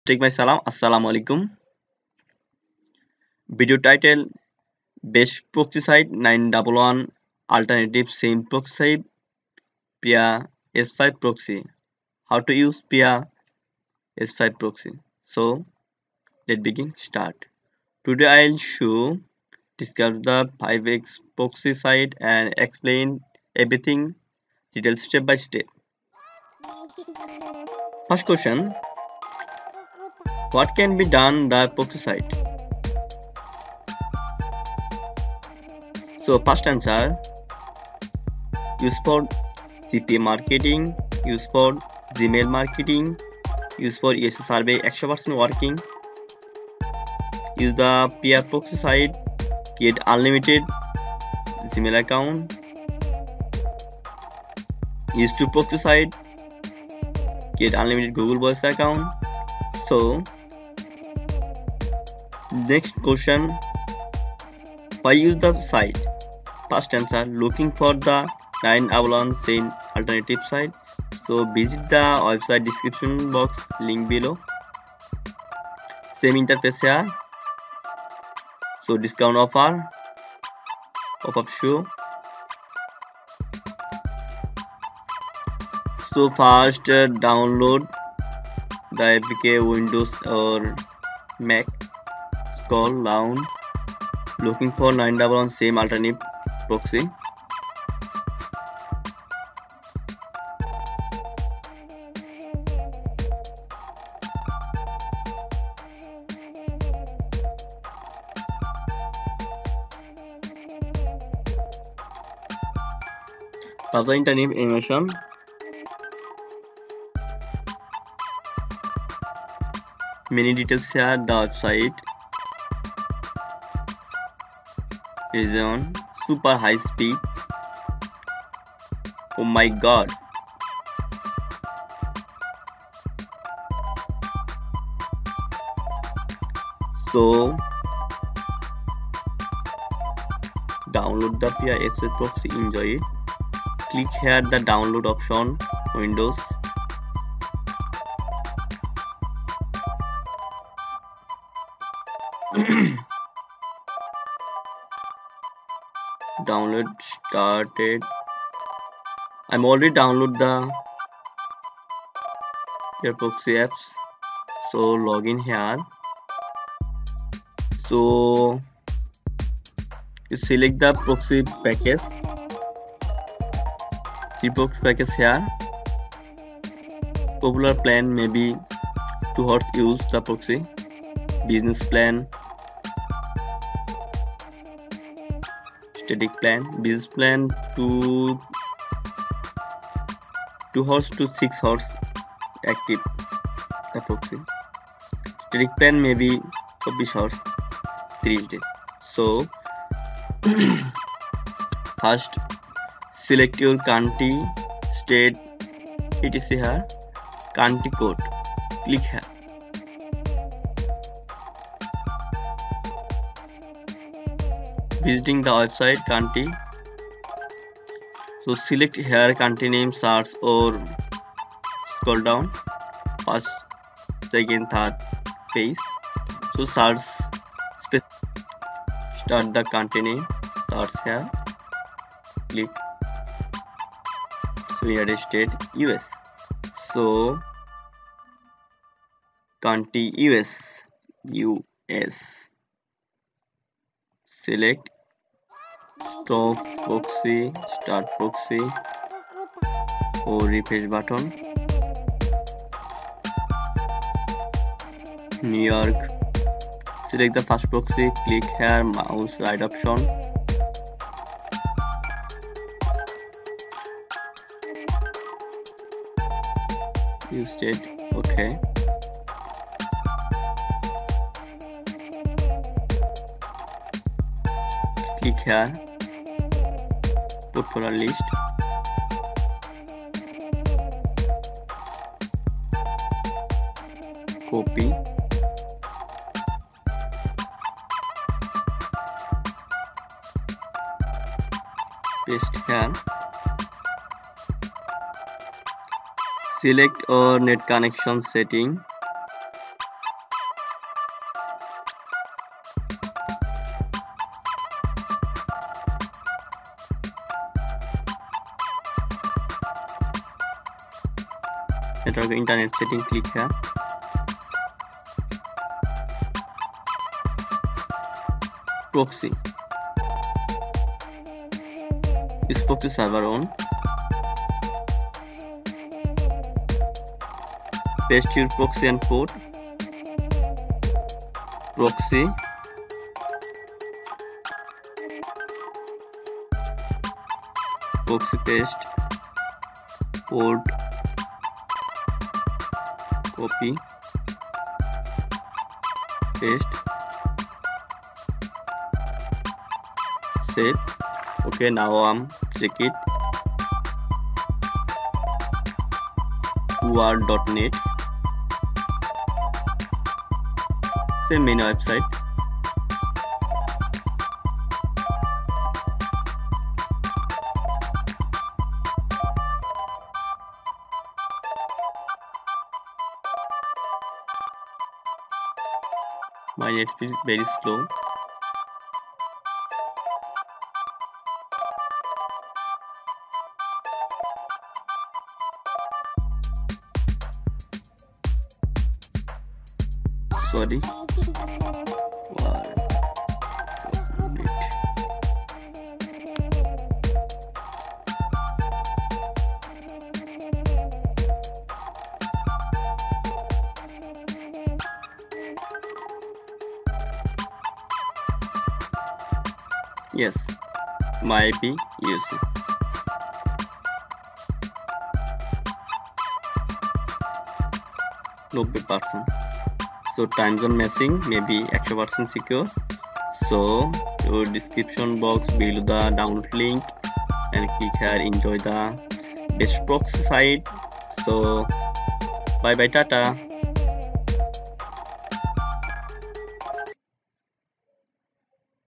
Best proxy site 911 re Alternative same site Pia S5 Proxy | How to use Pia S5 Proxy
Hi guys i will going to show you

My skype id s m sabbir :-  live:.cid.10cf7ae1af47ce8f

Today I will discuss pia s 5 poxy this site and explain everything in detail step by step .

 1 . What can be done with this proxy site?
Ans .

* Use For CPA Marketing

* Use For Gmail Marketing

* Use For USA Survey 100 % Working

* Use to Pia S5 Proxy Create Unlimited Gmail Usa Gmail Account

* Use to Pia S5 Proxy Create Unlimited Google Voice Account

 2 . Why use this site?
 Ans .

* Looking for a 911 S5 alternative

* Browse the internet anonymously

* 180+  Country / Region

* High speed server

* Dynamic IP address 50M+

3. How to use it
Ans . Download the Pia S5 Proxy Enjoy

Enjoy the best proxy site

S m Sabbir